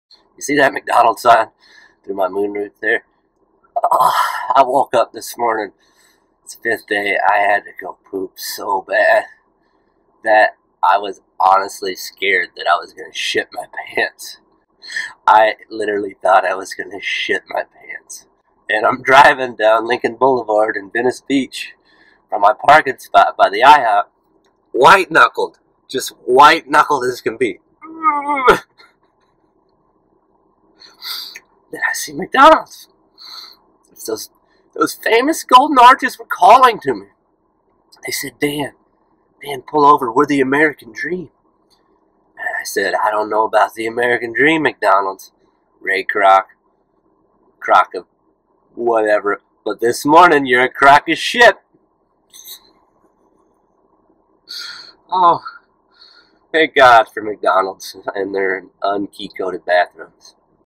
Almost shit my pants!
On Day 5
I woke up and had to go to the bathroom so bad I pulled into the nearest place I could find.
God Bless the Golden Arches of McDonalds!